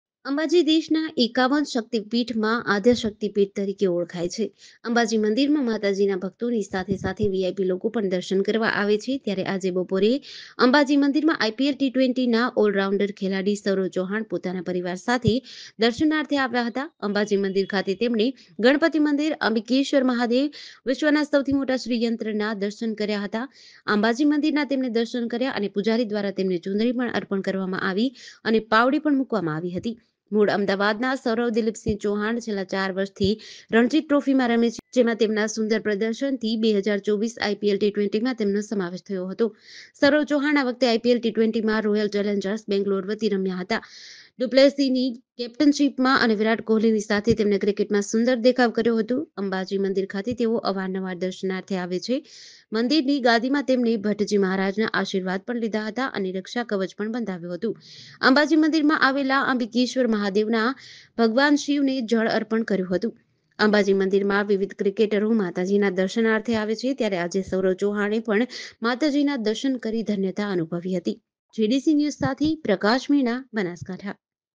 IPL T20ના ખેલાડીએ અંબાજી મંદિરમાં દર્શન કર્યા
હાલમાં જ ટીમ ઇન્ડિયાએ T20 વર્લ્ડ કપ જીતીને દેશનું નામ રોશન કર્યું